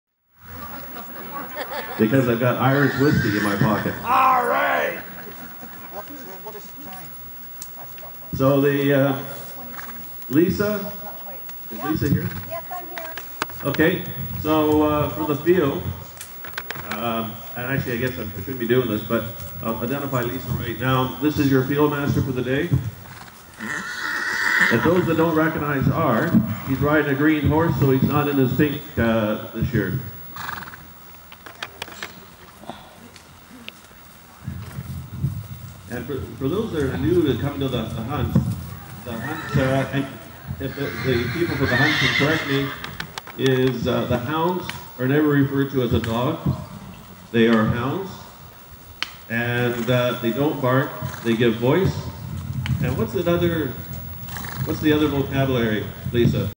15th Annual Fox Hunt
Kenan MacKenzie explains a few rules and how the 15th annual Opening Fox Hunt will unfold on Saturday, Nov. 8. The Sunshine Coast Equestrian Club hosted the event along with their guests from The Fraser Valley Hunt Club.